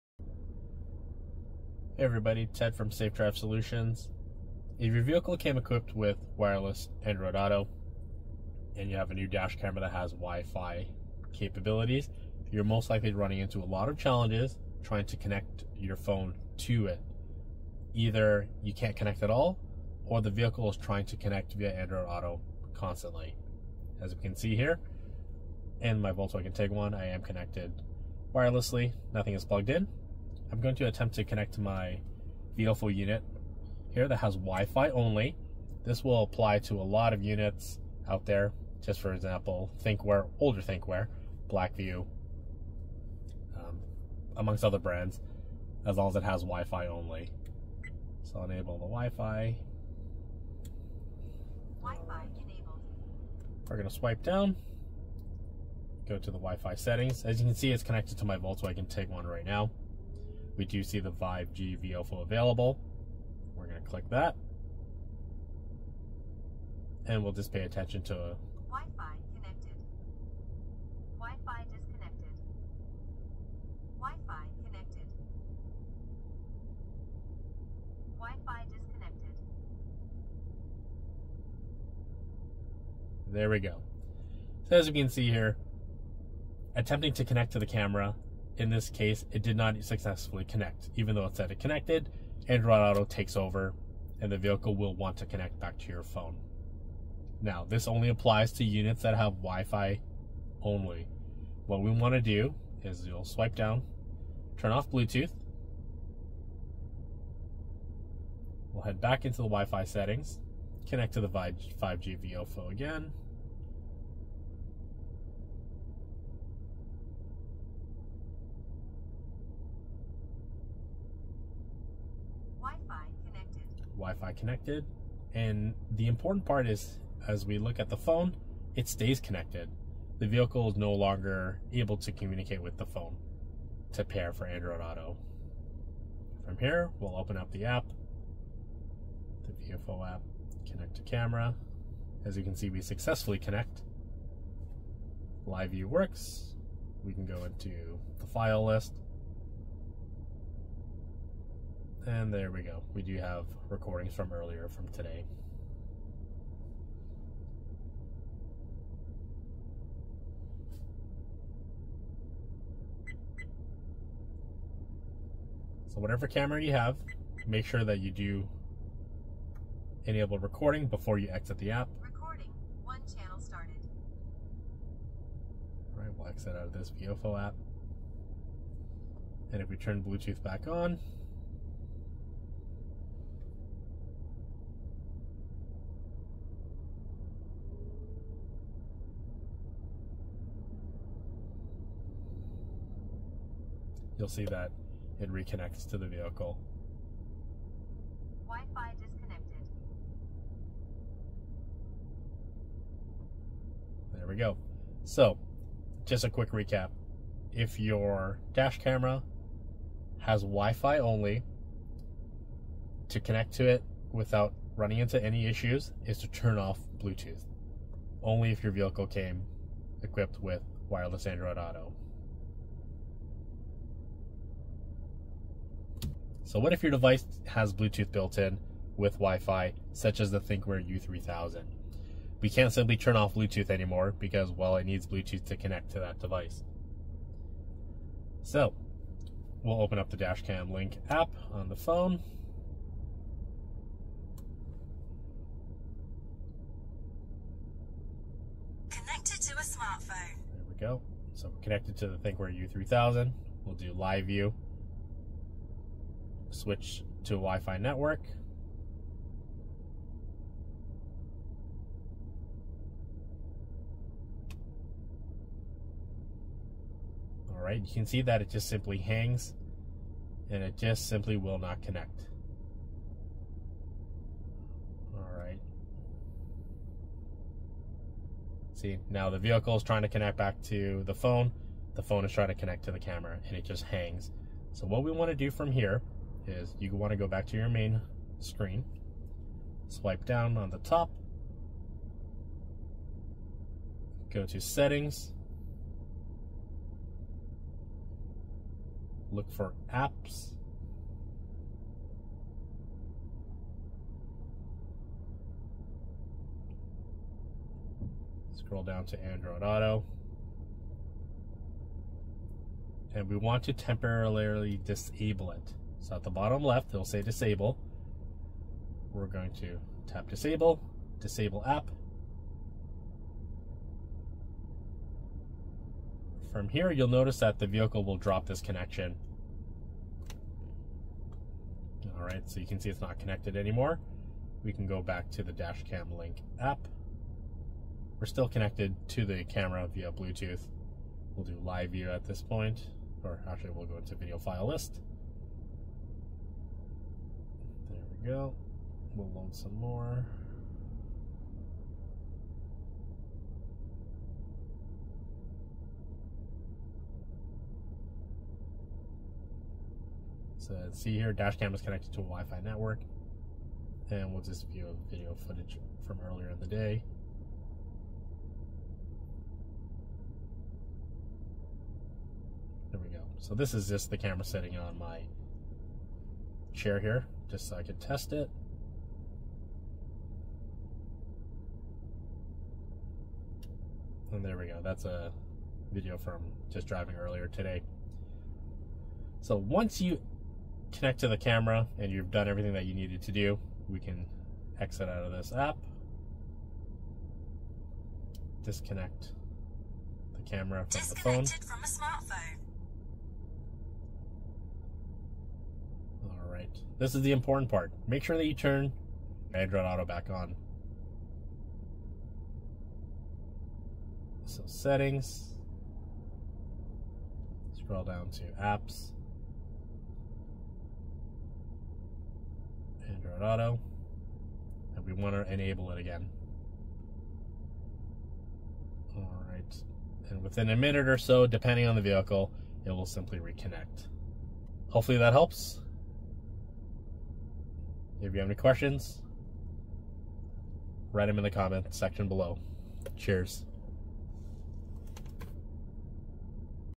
How to Connect Dash Cam with Wireless Android Auto
00:00 Introduction
00:30 Option 1 for connecting dash cam when wireless Android Auto is used
03:45 Option 2 for connecting Bluetooth + Wi-Fi enabled dash cams
04:45 How to turn off Android Auto on your android phone
06:46 How to turn Android Auto back on android phone
07:15 Conclusion

Safe Drive Solutions
We Help Drivers Provide Video Evidence

 Greater Vancouver Call or Text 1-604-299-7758

Greater Toronto Call or Text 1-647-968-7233